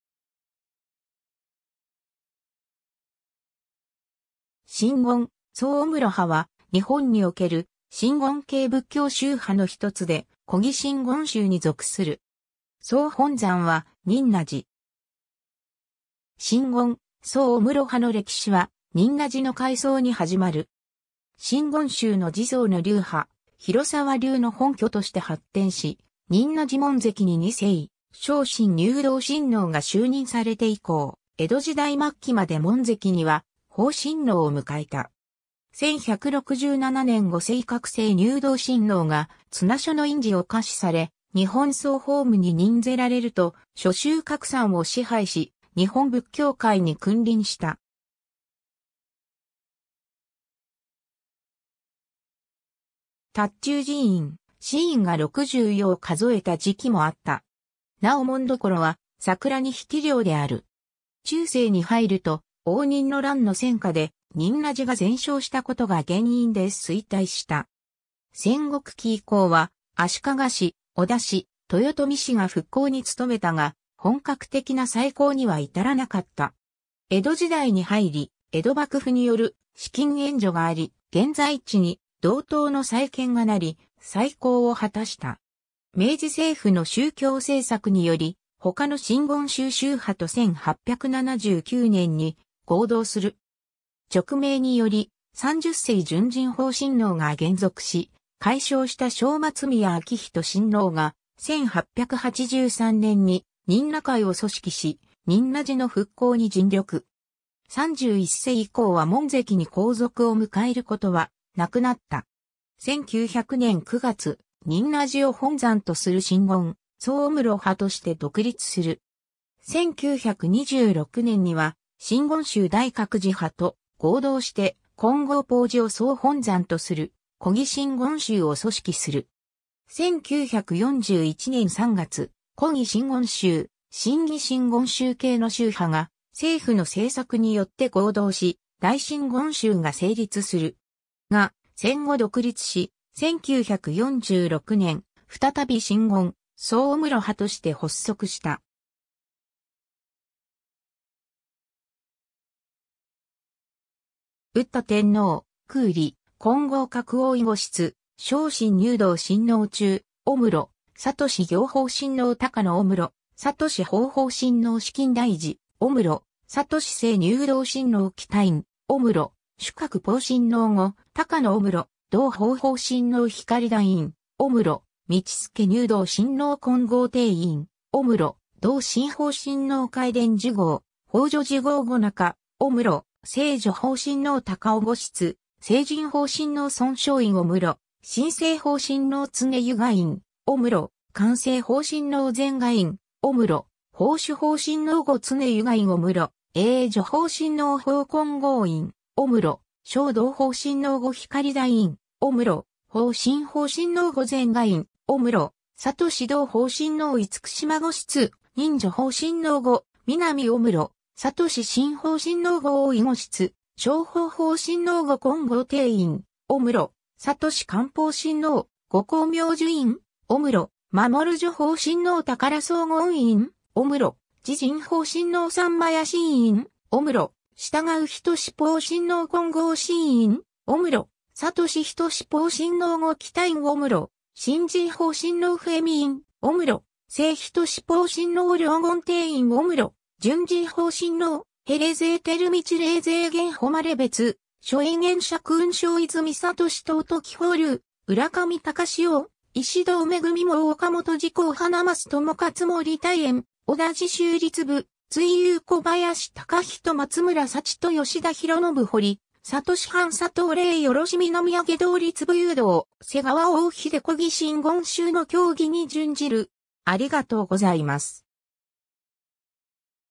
真言宗御室派
真言宗御室派（しんごんしゅうおむろは）は、日本における真言系仏教宗派のひとつで、古義真言宗に属する。
総本山は仁和寺。
 真言宗御室派の歴史は仁和寺の開創に始まる。
真言宗の事相の流派「広沢流」の本拠として発展し、仁和寺門跡に2世性信入道親王（大御室）が就任されて以降、江戸時代末期まで門跡には法親王（皇族）を迎えた。
1167年（仁安2年）5世覚性入道親王（紫金台寺御室）が綱所の印璽を下賜され、日本総法務に任ぜられると諸宗格山を支配し、日本仏教界に君臨した。
塔頭寺院・子院が60余を数えた時期もあった。
なお紋所は、桜に引両である。
 中世に入ると、応仁の乱の戦火で、仁和寺が全焼したことが原因で衰退した。
戦国期以降は足利氏・織田氏・豊臣氏が復興に努めたが、本格的な再興には至らなかった。
 江戸時代に入り、江戸幕府による資金援助があり、現在地に堂塔の再建がなり、再興を果たした。
 明治政府の宗教政策により、他の真言宗宗派と1879年（明治12年）に合同する。
勅命により、30世純仁法親王（楞厳定院御室）が還俗し改称した小松宮彰仁親王が、1883年（明治16年）に仁和会を組織し、仁和寺の復興に尽力。
31世以降は門跡に皇族を迎えることは無くなった。
 1900年（明治33年）9月、仁和寺を本山とする真言宗御室派として独立する。
 1926年（大正15年）には（真言宗高野派（総本山金剛峯寺）・真言宗大覚寺派（大本山大覚寺））と合同して金剛峯寺を総本山とする古義真言宗を組織する。
 1941年（昭和16年）3月、古義真言宗・新義真言宗系の宗派が政府の政策によって合同し、大真言宗が成立する。
が、戦後独立し、1946年（昭和21年）、再び真言宗御室派として発足した。
 宇多天皇（光孝天皇第7皇子）：空理・金剛覚 大御室（三条天皇第4皇子）：性信入道親王 中御室（白河天皇第3皇子）：覚行法親王（覚念） 高野御室（白河天皇第4皇子）：覚法法親王（真行） 紫金台寺（しこんだいじ）御室（鳥羽天皇第5皇子）：覚性入道親王（信法） 喜多院御室（後白河天皇第2皇子）：守覚法親王（守法） 後高野御室（後白河天皇第8皇子）：道法法親王（尊性） 光台院御室（後鳥羽天皇第2皇子）：道助入道親王 金剛定院御室（後高倉院第2皇子）：道深法親王 開田准后（藤原道家第5子）：法助准后 後中御室（後嵯峨天皇第6皇子）：性助法親王 高雄御室（後深草天皇第4皇子）：性仁法親王 尊勝院御室（後深草天皇第6皇子）：深性法親王 常瑜伽院御室（伏見天皇第3皇子）：寛性法親王 禅河院御室（後伏見天皇第3皇子）：法守法親王 後常瑜伽院御室（後光厳天皇第5皇子）：永助法親王（空助） 法金剛院御室（後小松天皇猶子）：承道法親王 後光台院御室（後花園天皇猶子）：法深法親王（弘覚・静覚） 後禅河院御室（後柏原天皇第2皇子）：覚道法親王 厳島御室（伏見宮貞敦親王第4子）：任助法親王 後南御室（後陽成天皇第1皇子）：覚深法親王 後大御室（後水尾天皇第3皇子）：承法法親王（性承） 後金剛定院御室（霊元天皇第2皇子）：覚観法親王（覚恕・覚隆） 後光明寿院御室（京極宮文仁親王第2子）：守恕法親王 宝荘厳院御室（中御門天皇第4皇子）：慈仁法親王 三摩耶心院御室（中御門天皇第6皇子）：遵仁法親王 金剛心院御室（有栖川宮職仁親王第3子）：覚仁法親王 後喜多院御室（閑院宮典仁親王第2子）：深仁法親王 不壊身院御室（有栖川宮職仁親王第11子）：済仁法親王 楞厳定院御室（伏見宮邦家親王第5子）：純仁法親王（勅命により復飾、還俗して小松宮彰仁親王と改称する） 冷泉照道 冷泉玄誉 別處栄厳 釈雲照 泉智等 土宜法龍 浦上隆応 石堂恵猛 岡本慈航 花桝智勝 森諦圓 小田慈舟 立部瑞祐 小林隆仁 松村祐澄 吉田裕信 堀智範 佐藤令宜 南揚道 立部祐道 瀬川大秀 古義真言宗の教義に準じる。